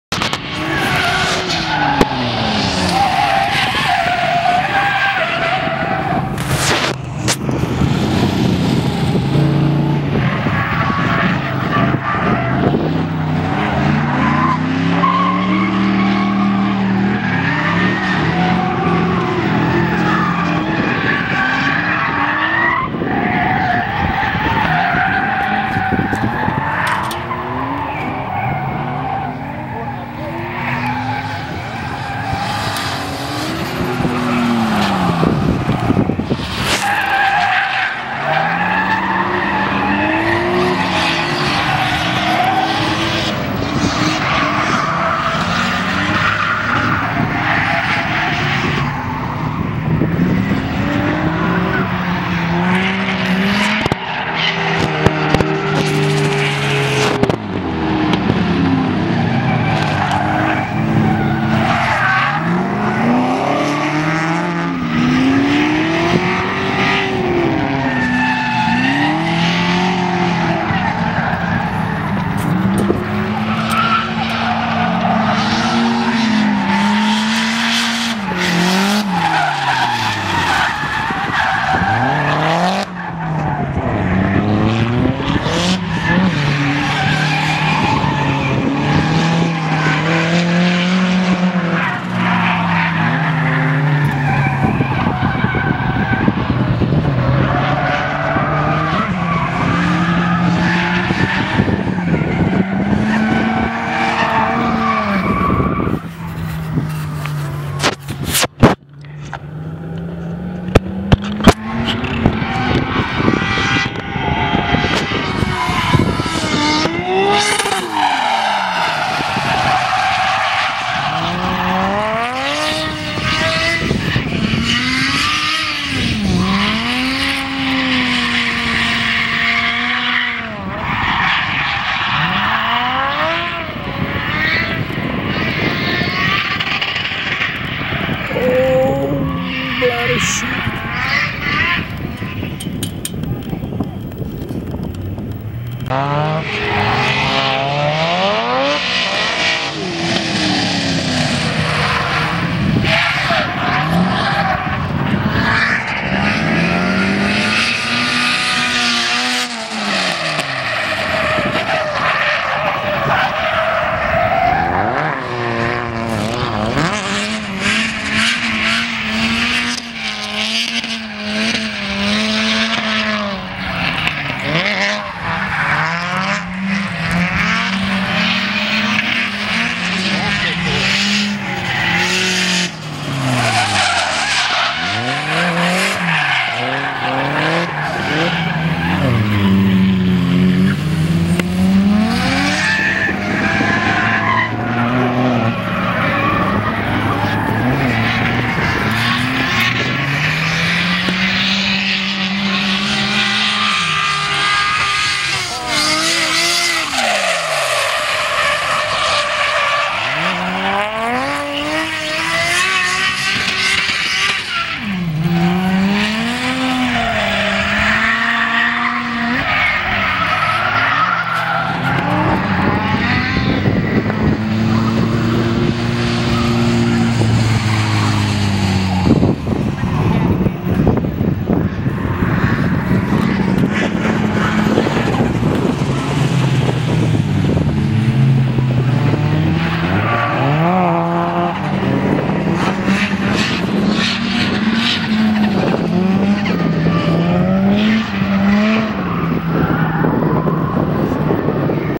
Drift Prac 2nd dec 2011, V8 Race - Night.wmv
Drift Prac 2nd dec 2011, V8 Race - Night

Some more night footage!